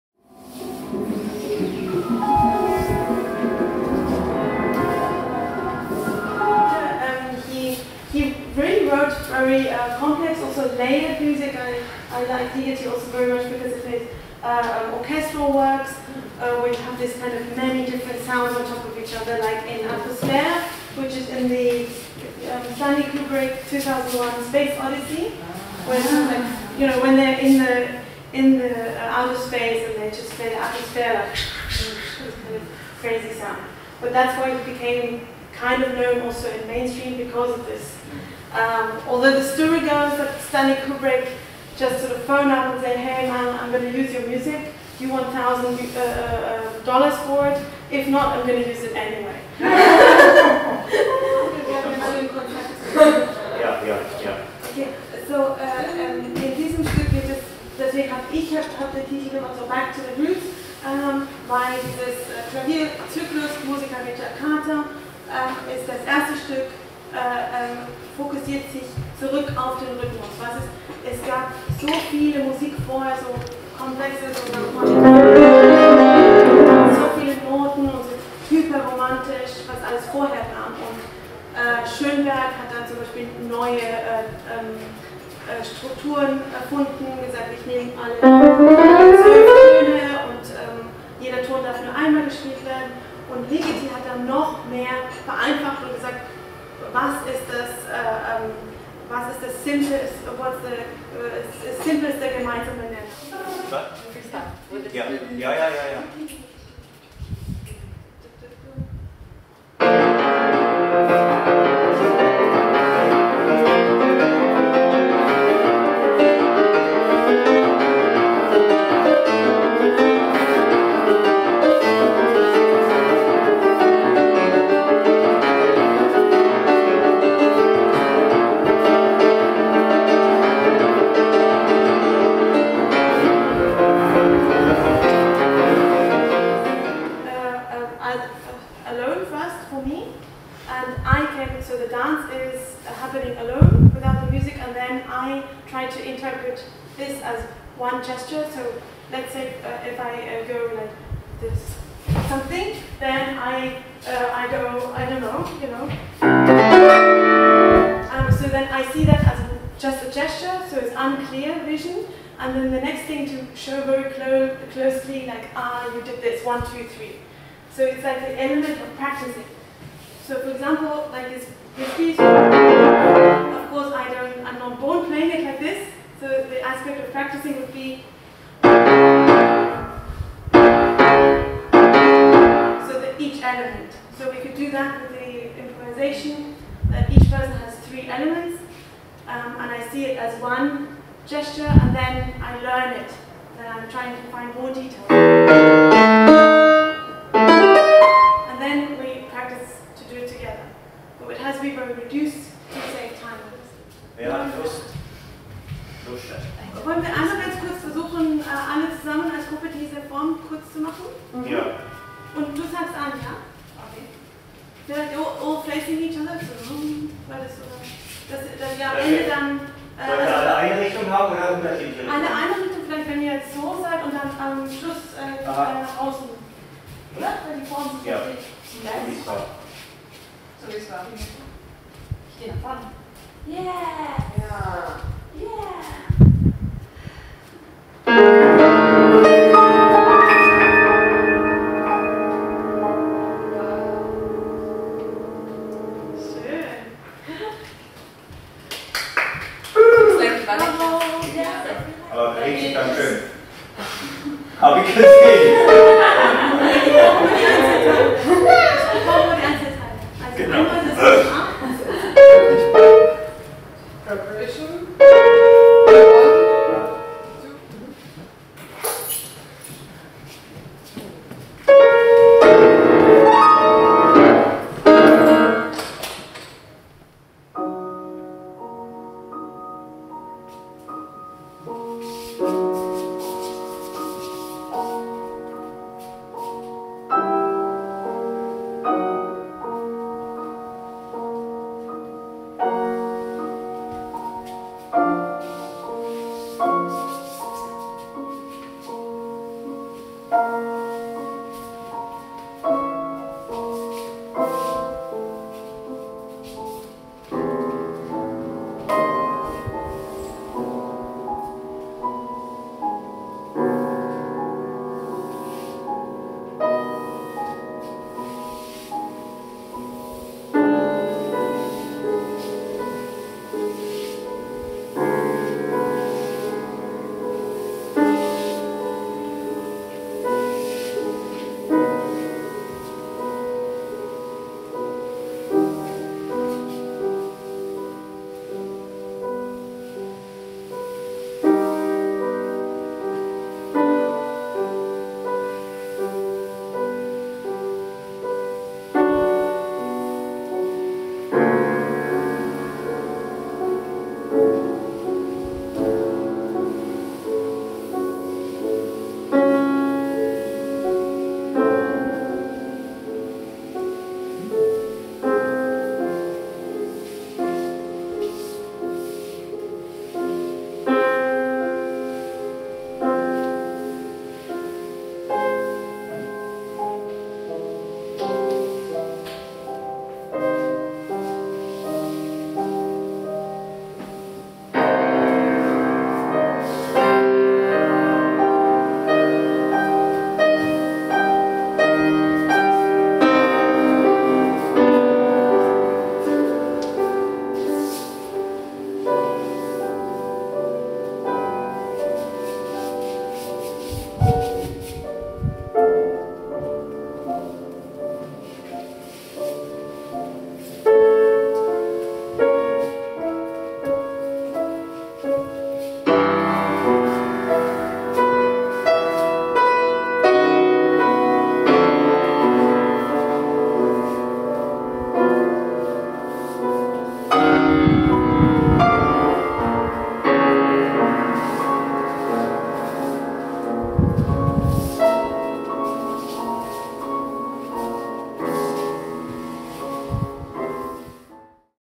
Dance Workshop July 2019, Jean Beers (new music) and Ingo Reulecke (dance/choreography)
at Ernst Busch Dance Academy in Berlin
with MA choreography students

Copyright with the Ernst Busch Dance Academy, Ingo Reulecke, Jean Beers and the students. PLEASE DO NOT SHARE WITHOUT PERMISSION FROM JEAN BEERS ! Thank you.